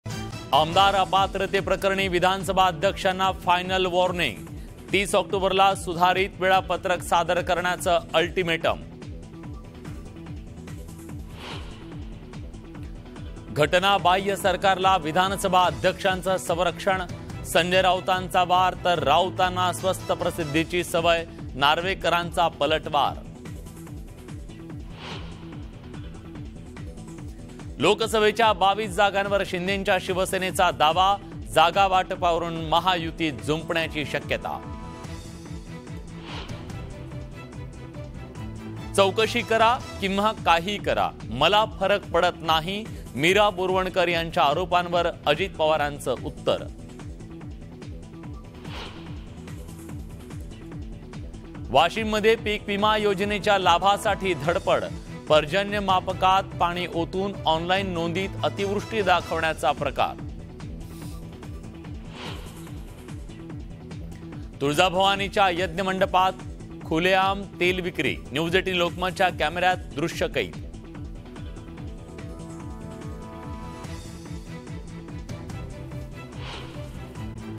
Marathi News Headlines | 7 PM News Today | Latest Maharashtra News | News18 Lokmat | Oct 17, 2023
Supreme Court Hearing on MLA Disqualification : आमदारांच्या अपात्रता सुनावणीमध्ये मोठी अपडेट समोर आली आहे. सुप्रीम कोर्टानं पुन्हा एकदा विधानसभा अध्यक्षांच्या कार्यपद्धतीवर नाराजी व्यक्त केली आहे. गेल्या सुनावणीमध्ये सोमवारपर्यंत सुधारित वेळापत्रक सादर करा असे आदेश कोर्टानं दिले होते. मात्र सुधारित वेळापत्रक सादर करण्यात न आल्यानं सुप्रीम कोर्टानं नाराजी व्यक्त करत अध्यक्षांच्या कार्यपद्धतीवर ताशेरे ओढले आहेत.